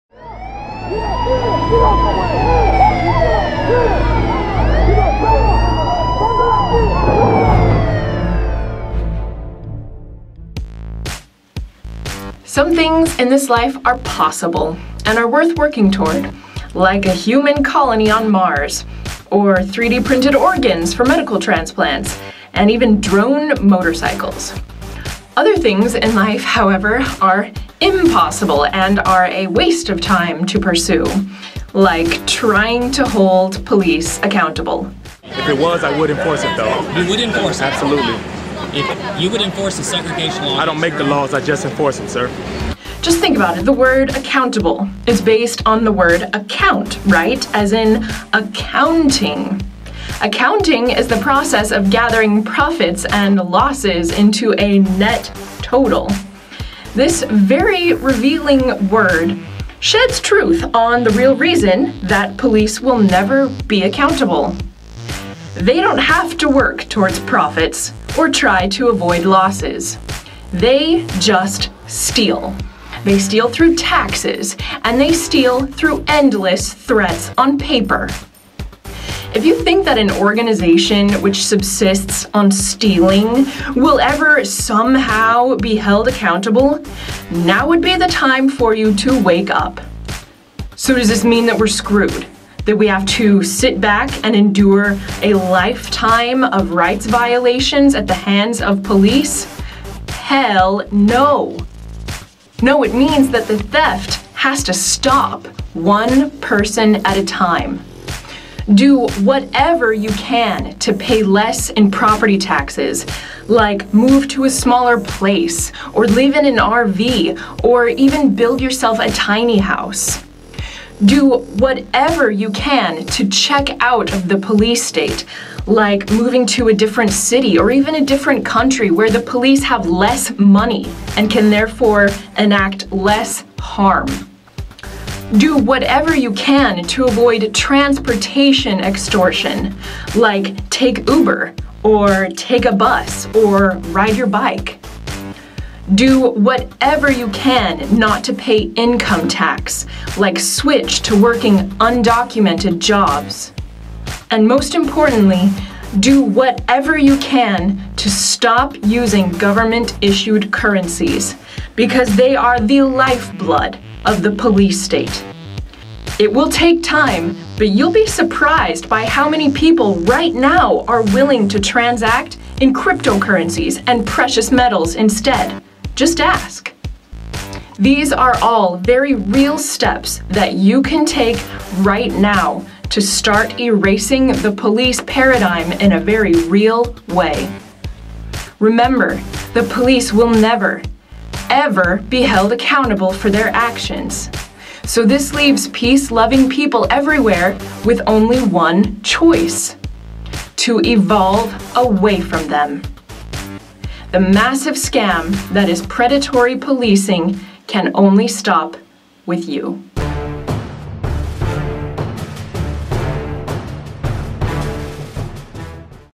Police Accountability Will NEVER Happen
Police accountability will never happen. Why? It all comes down to what it means to be accountable. Unpack the words "police accountability" and it will be clear to you why it's an impossibility.